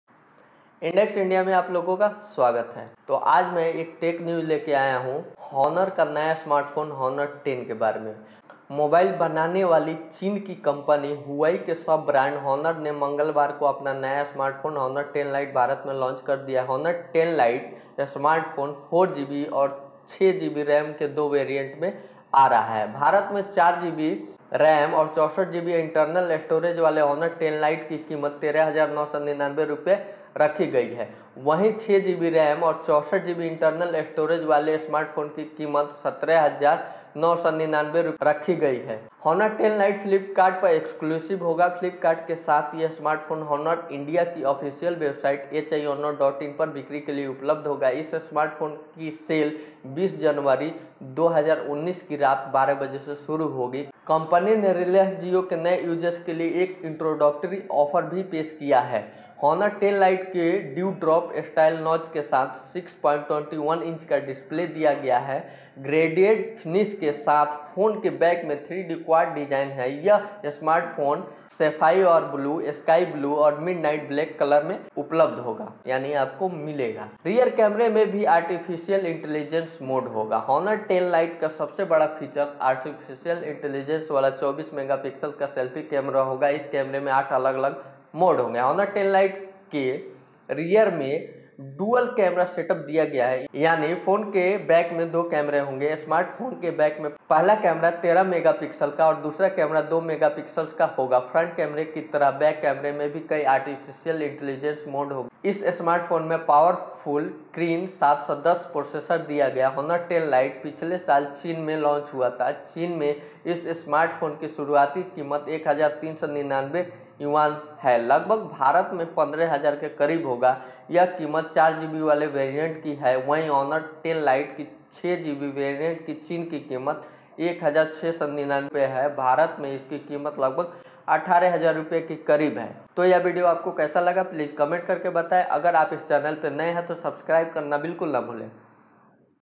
The Honor 10 Lite smartphone in India INDEX INDIA
The Honor 10 Lite smartphone with its AI-enabled features, such as the 24 MP AI Selfie Camera, AI-enhanced Calls, and HiVision Feature will elevate your senses for a delightful smartphone experience. Oh, and not to forget, this phone’s Super Night Scene Mode paves the way for brilliant photographs in the night. It is powered by the Kirin 710 Chipset which ensures a seamless performance.